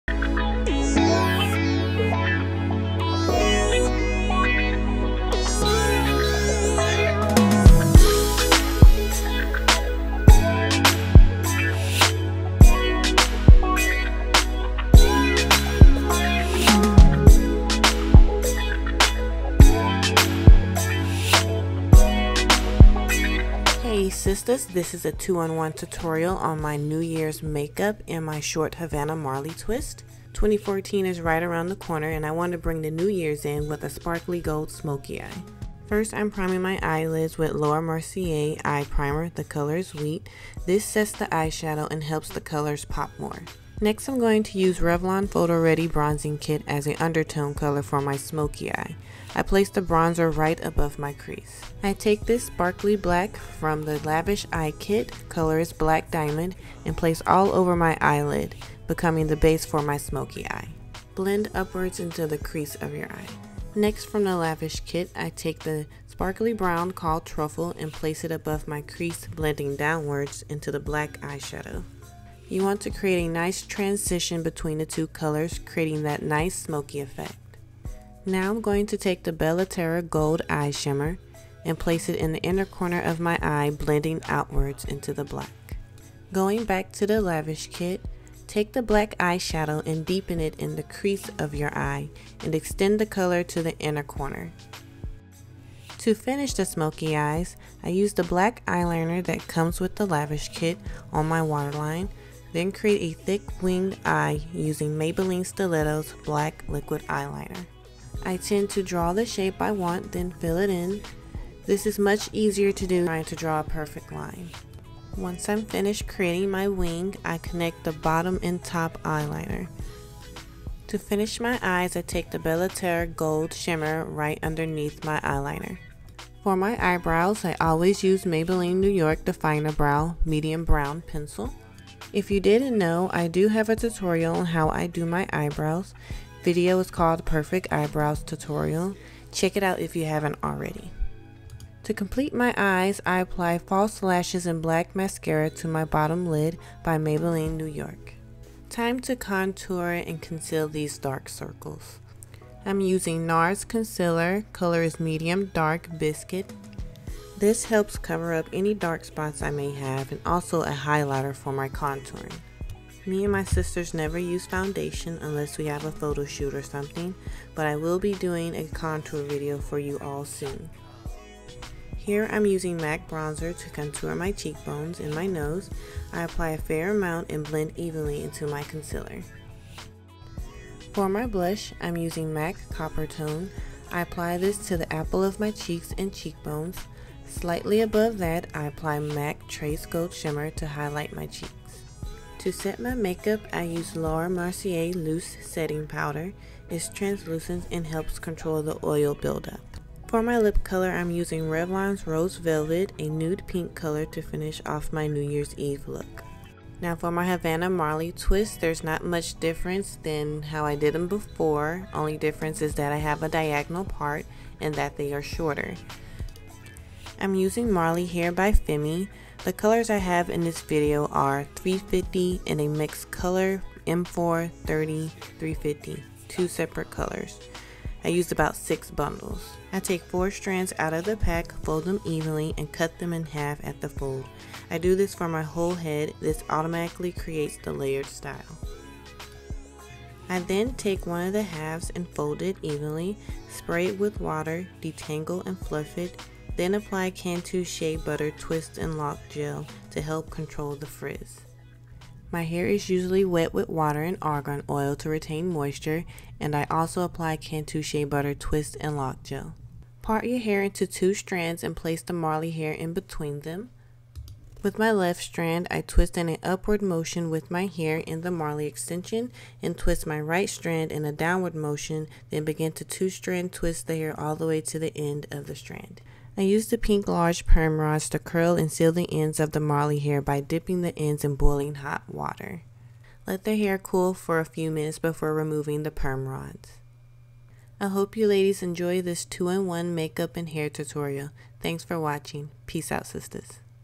New Year's Eve Glittery Smokey Eye Make-Up & DIY Short Marley Twist Hair Tutorial
A two in one make up and hair tutorial. A New Years Eve night on the town sparkly gold smokey eye with pink nude lips. A quick how to Havanna Marley Twist "short bob style". I have a previous tutorial on how the longer Havanna twist. I used 6 bags for this style in the video, takes 6-8 hours to complete hair, last a good 3 weeks.You can easily find marley hair at your local beauty supply store for 5.99, brand I use is Femi, red color in this video is a mixture of 350 and M4/30/350.
Eyeshadow make up kit used: Lavish, find at Sephora!